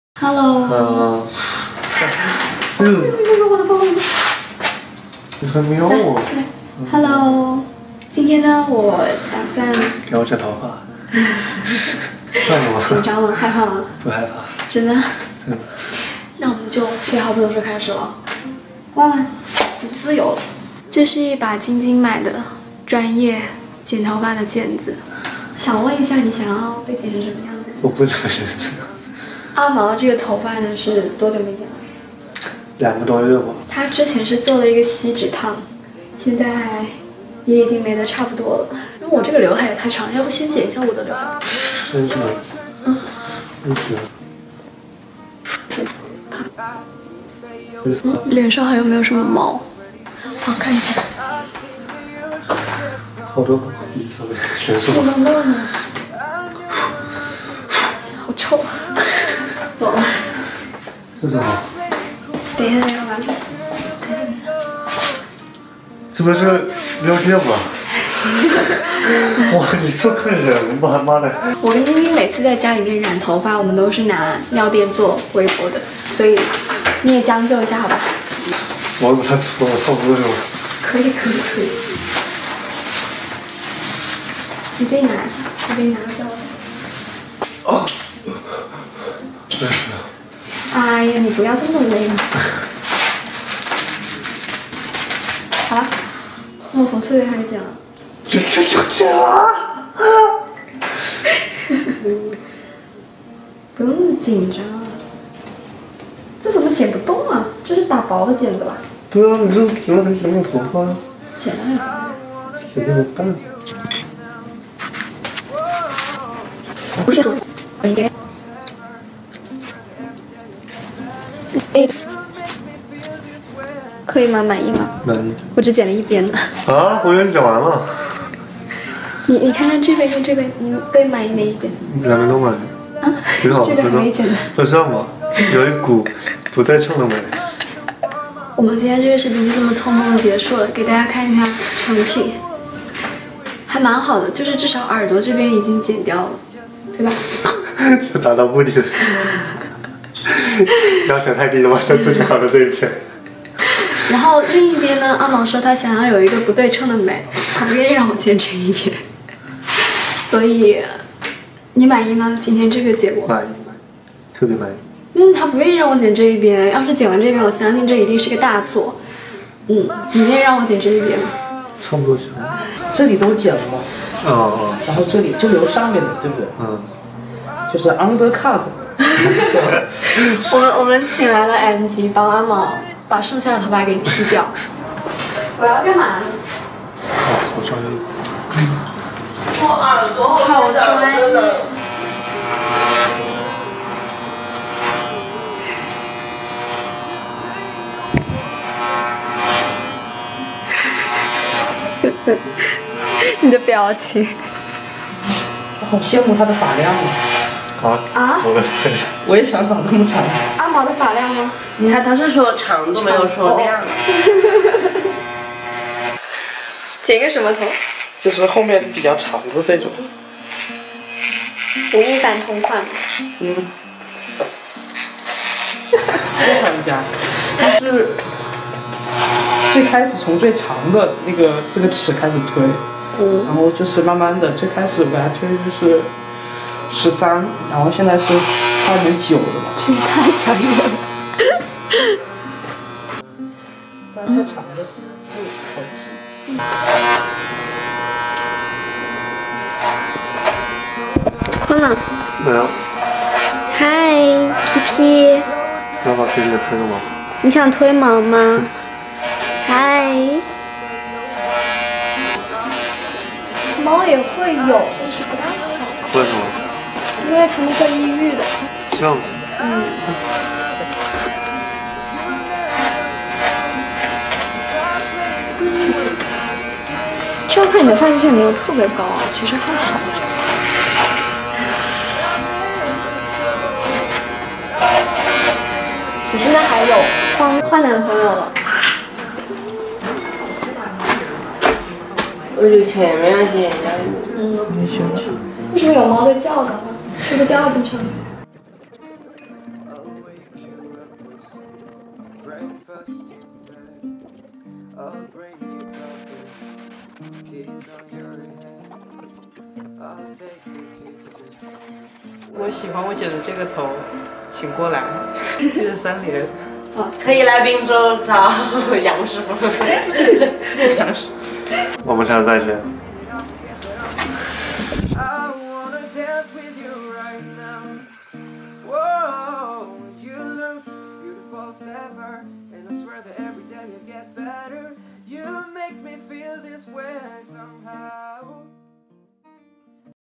给男朋友剪头发！本以为是王者，结果是个青铜｜男朋友也太乖了吧#留学情侣日记 ｜ 疫情宅家
美国疫情期间宅家宅出了好多新技能，我竟然能帮阿毛剪头发了！
不过效果真的太糟糕了哈哈哈最后还是请了朋友tony老师anji帮阿毛推了个头
内容有点点水但是拍的时候真的一直都是笑声连连哈哈哈